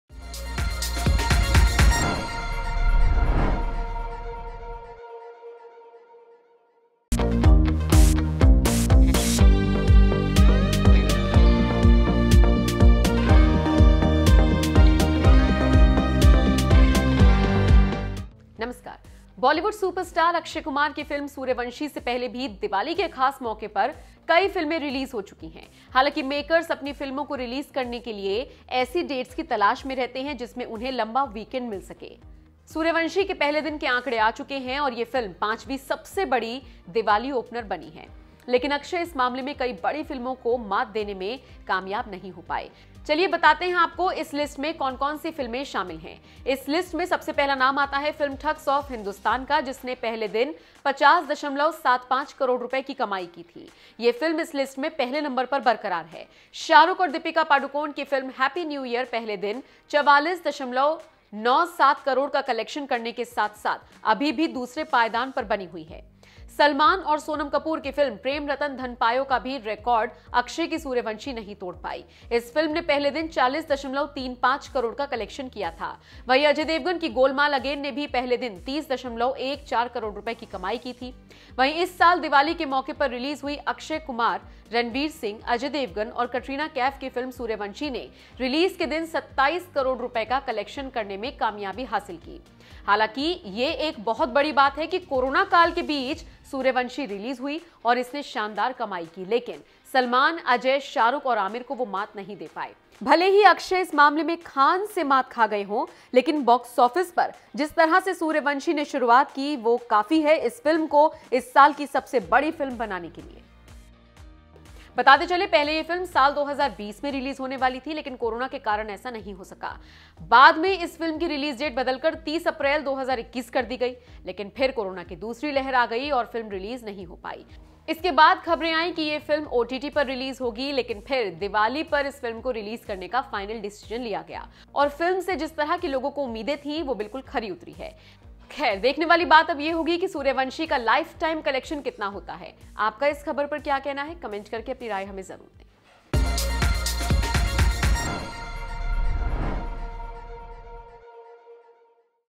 Akshay की Sooryavanshi नहीं छू पाई Salman, SRK और Aamir का ये Record
बॉलीवुड सुपरस्टार अक्षय कुमार की फिल्म सूर्यवंशी से पहले भी दिवाली के खास मौके पर कई फिल्में रिलीज हो चुके हैं... हालांकि मेकर्स अपनी फिल्मों को रिलीज करने के ऐसी डेट्स की तलाश में रहते हैं, जिसमें उन्हें लंबा वीकेंड मिल सके..
vinay
prashant